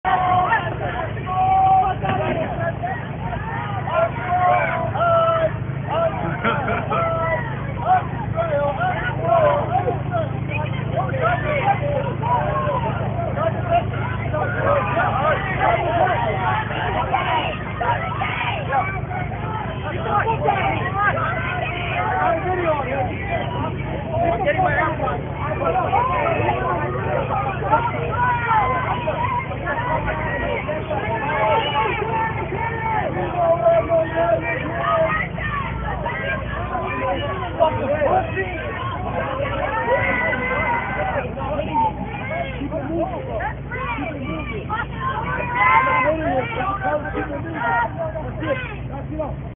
Man gets Assaulted by Islamic Protesters outside Met Gala
Man gets assaulted by Islamic protesters for recording their protest with his phone.  Video was taken outside the Metropolitan Museum's Met Gala last night, after police pushed the protest into Central Park.  EXCLUSIVE VIDEO FOR HOT ZONE.